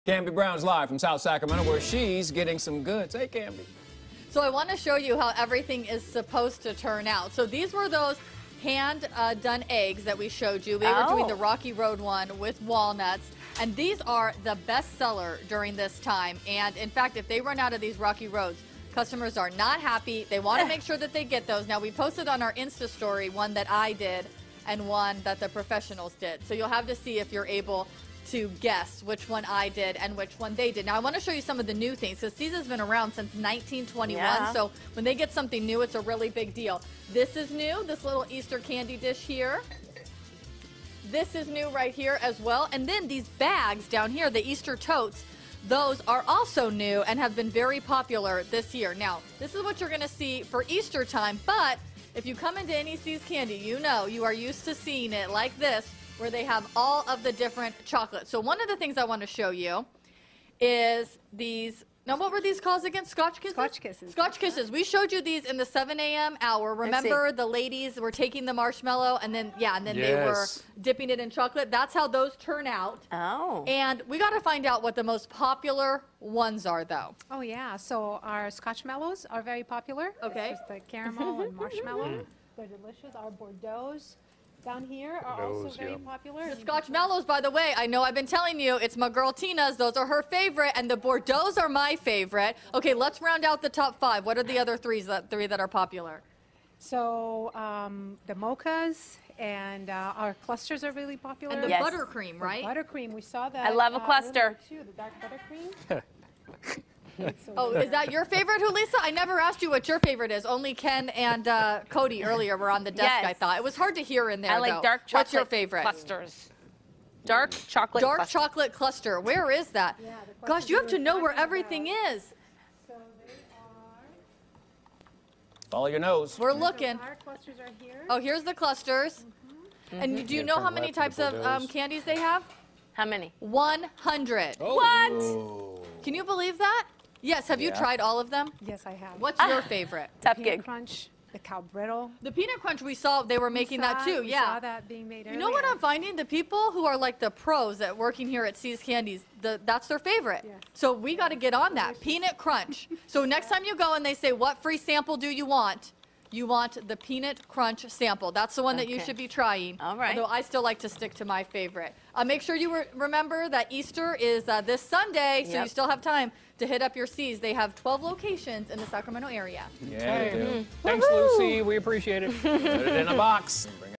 See’s Candies Pt. 3
Cambi Brown joins us from the See’s Candies in South San Francisco where she’s showing us how to shop this Easter!